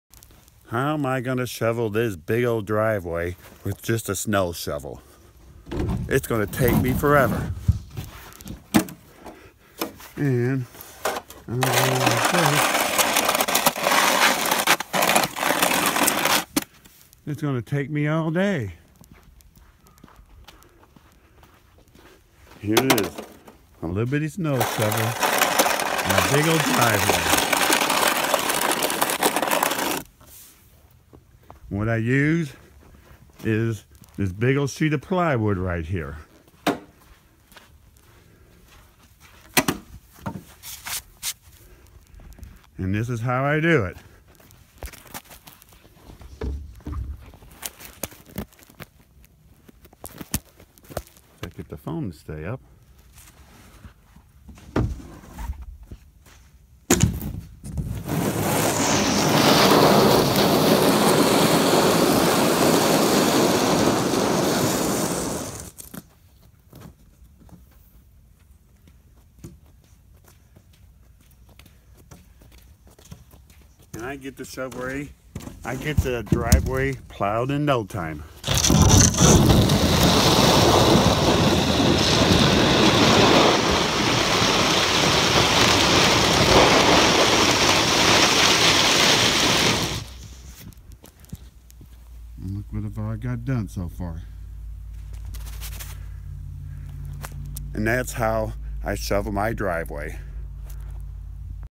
Hack for shoveling snow off driveway with sheet of plywood.
My hack for shoveling my driveway with a sheet of plywood instead of a snow shovel. This video is being managed exclusively by Newsflare. or email